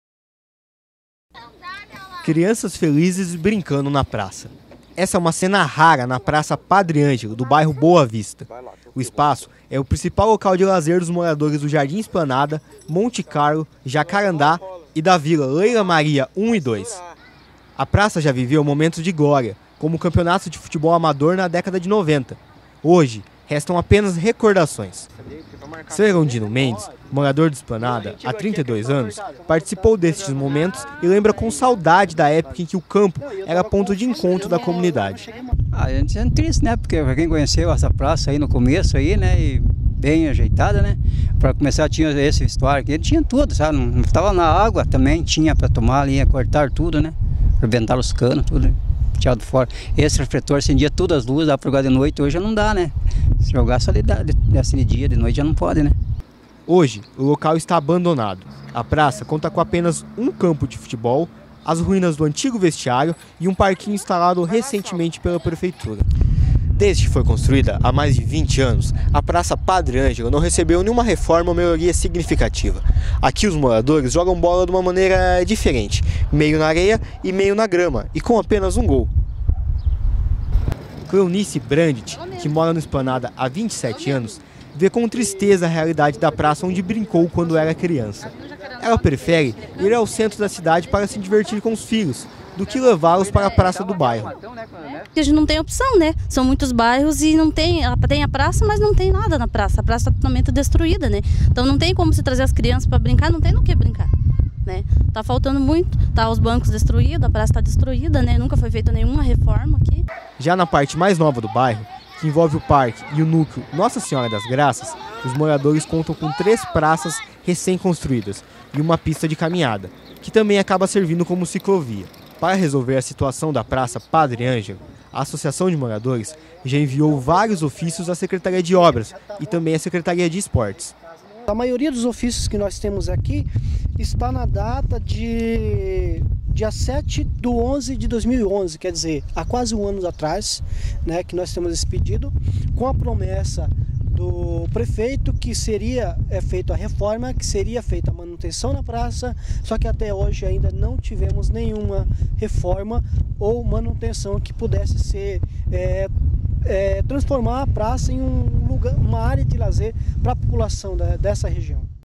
Praça do Jardim Esplanada está em condições precárias
A Praça Padre Ângelo Fernandez Caballero era e continua sendo o principal espaço de lazer da região e o que tem mais reclamações dos moradores. Construída em 1990, a praça conta apenas com uma quadra de futebol, que não se sabe mais se é de saibro ou de grama. Além disso, o vestiário está em ruínas, o alambrado destruído em várias partes e a iluminação não existe mais.